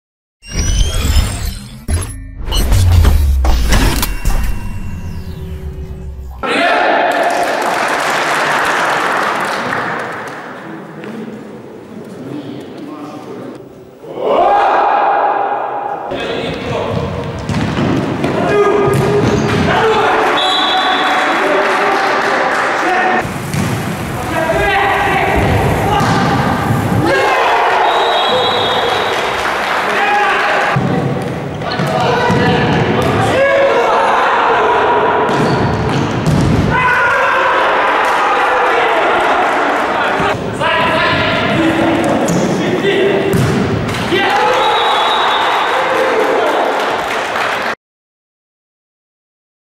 "Метекс" - "Дилижанс" 2:2. ОБЗОР матча
Чемпионат Кременчуга по футзалу. 4 лига. "Метекс" - "Дилижанс" 2:2.